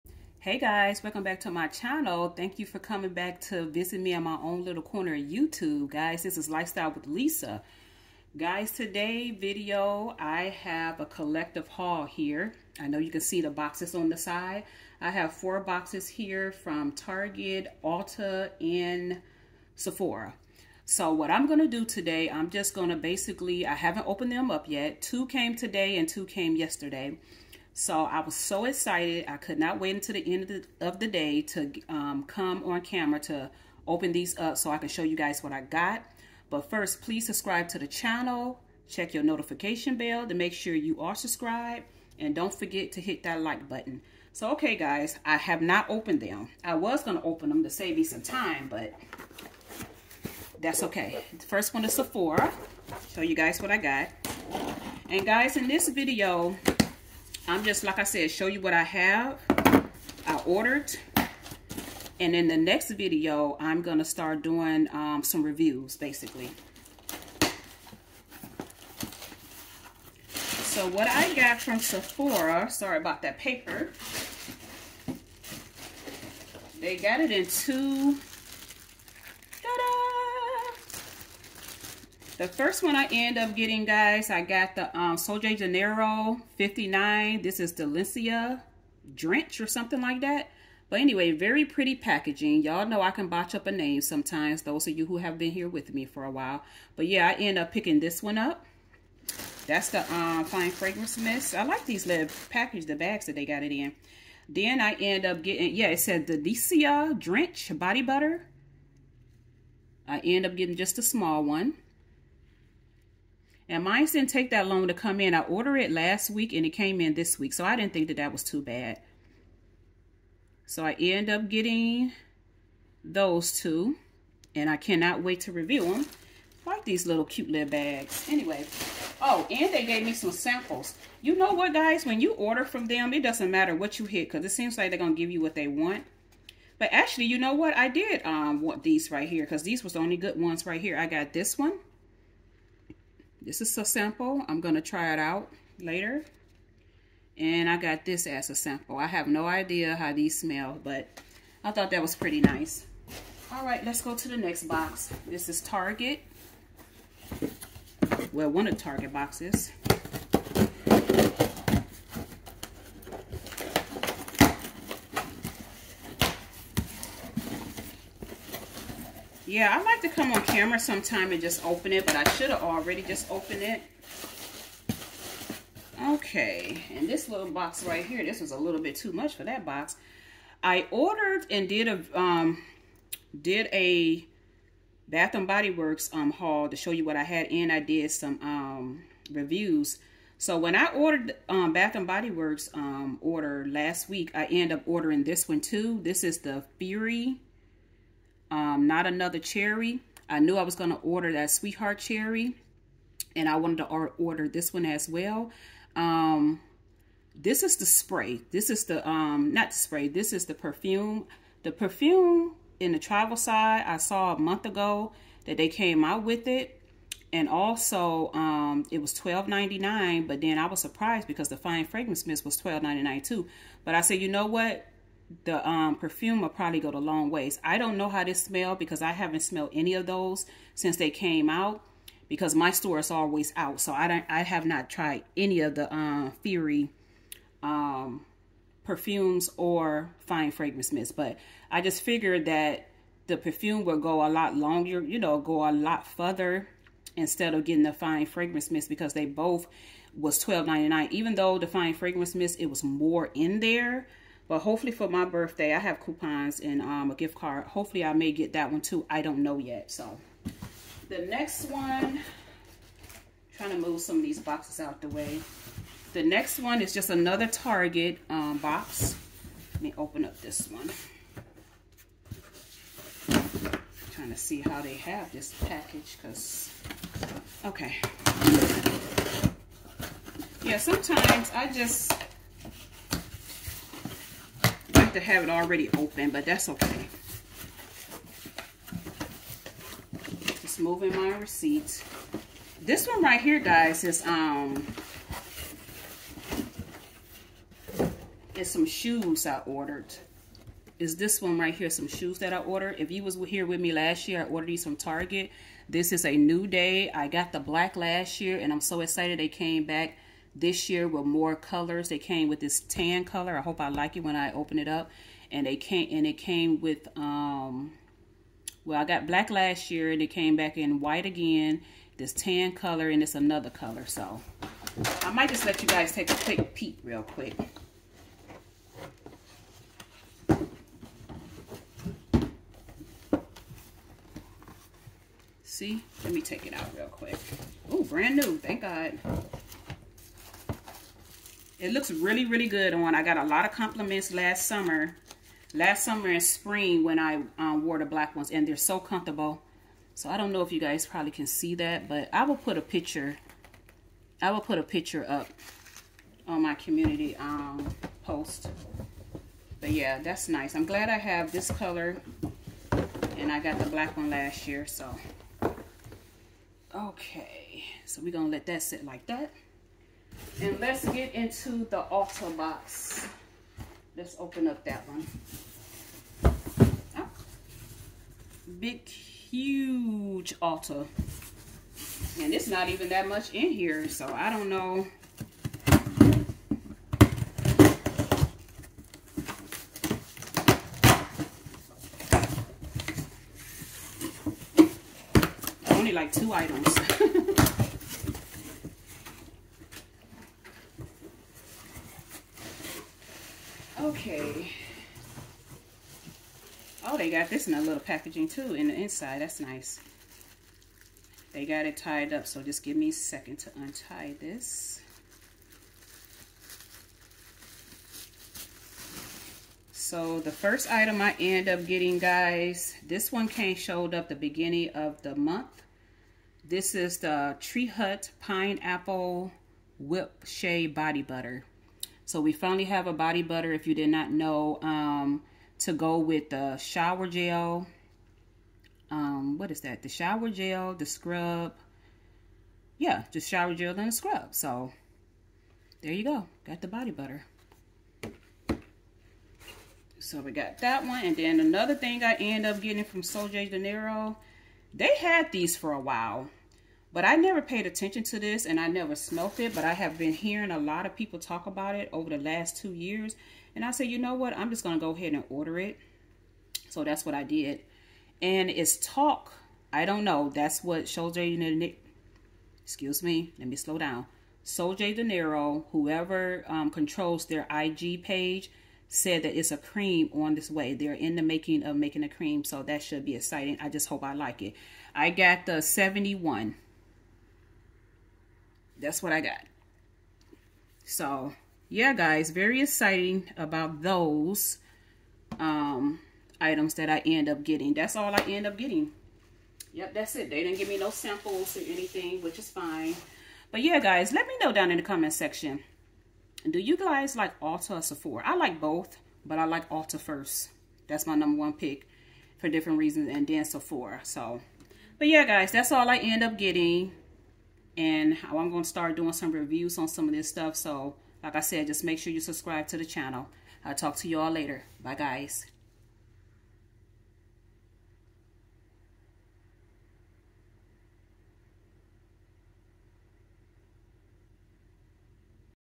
NEW COLLECTIVE HAUL * NEW BODY CARE * 2024
This video was recorded on January 31, 2024

Hello everyone, welcome back to my channel thank you for coming to visit me in my own Lil corner of YouTube in this video I am just doing a collective haul on different products I bought at Target, Ulta, & Sephora thanks for watching ❤️